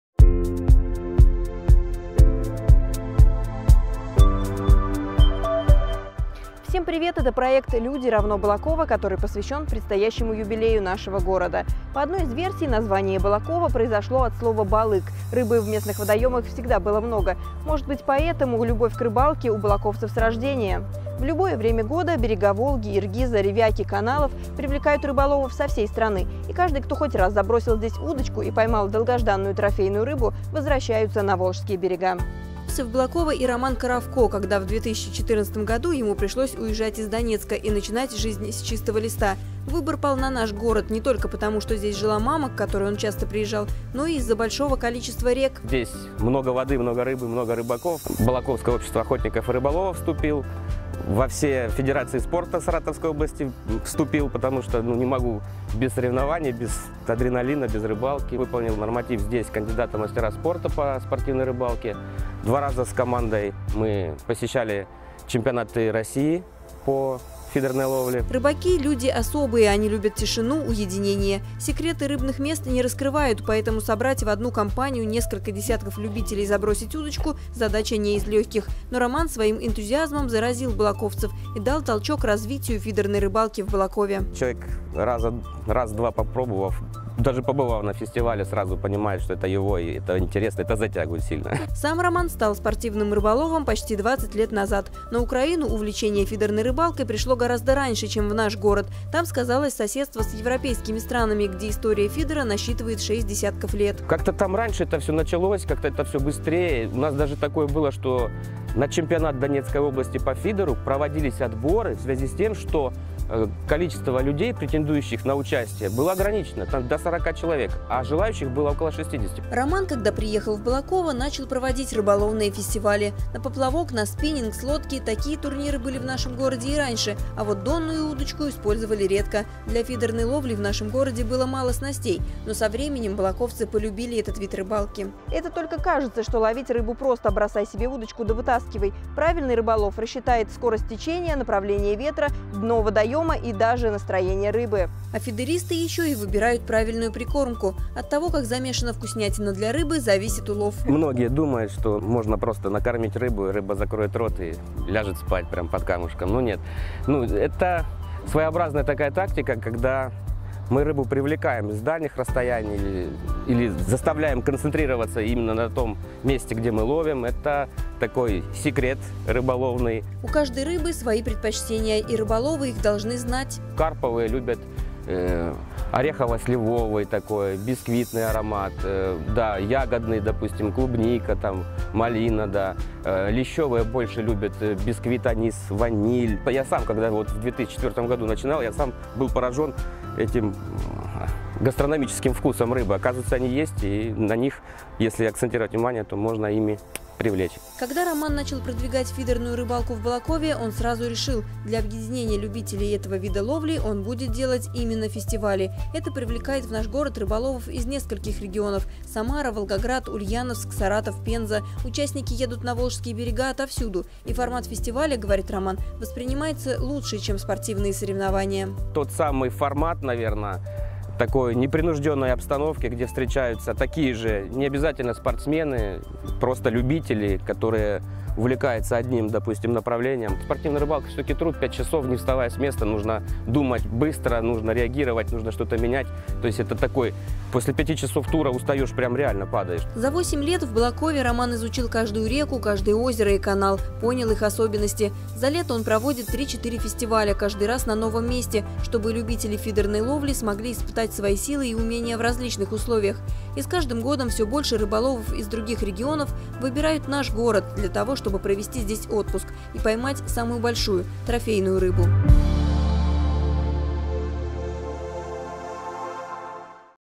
ЛЮДИ=Балаково #21 (Роман Коровко, рыболов-спортсмен)
«Балаковский репортер» и МЗ Балаково продолжают серию сюжетов о балаковцах, которые становятся движущей силой для окружающих
Приехать в Балаково, начать жизнь с чистого листа и объединить местных рыбаков в Любительскую фидерную лигу Саратовской области — это сумел спортсмен-рыболов Роман Коровко. Попав в первую волну беженцев с Украины в 2014 году, Роман не опустил руки, а смог реализовать свои планы в нашем городе. Его природный оптимизм и любовь к фидерной ловле передались балаковским рыбакам, и они охотно стали осваивать азы ловли рыбы донной удочкой. Сейчас Роман со своей инициативной группой проводит по 3-4 фестиваля за летний сезон, на них съезжаются фидеристы из нескольких регионов, а призы предоставляет Металлургический завод Балаково. Об объединении единомышленников, культуре правильной рыбалки и бережном отношении к водным ресурсам — в нашем новом сюжете проекта «ЛЮДИ=Балаково», который приурочен к предстоящему юбилею города.